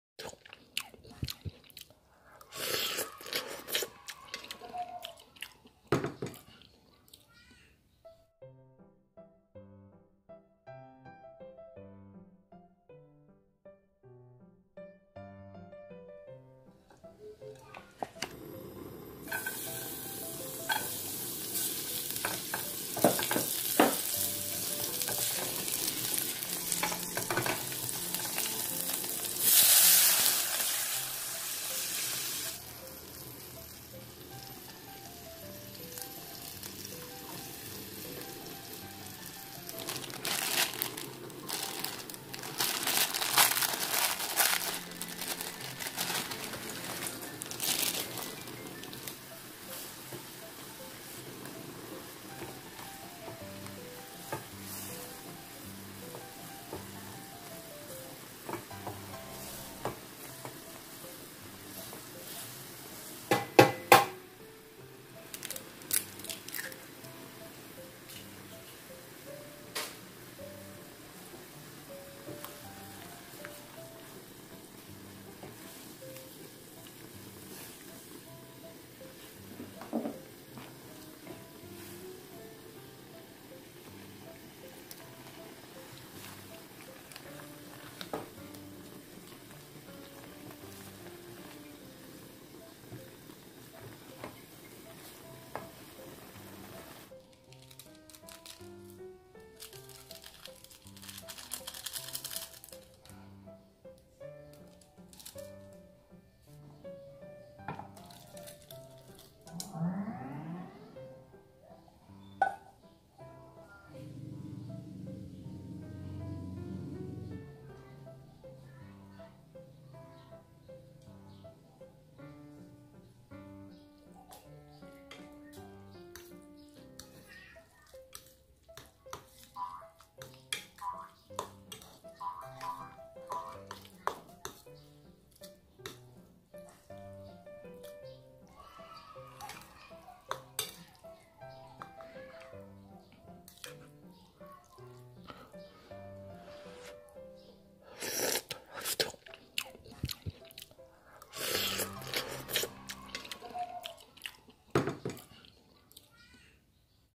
Masak Indomie Kari ayam - Kayak gini aja udah Enak
Kreasi Masak mie di rumah..pakai bahan-bahan yg ada di rumah aja bun...sedaap..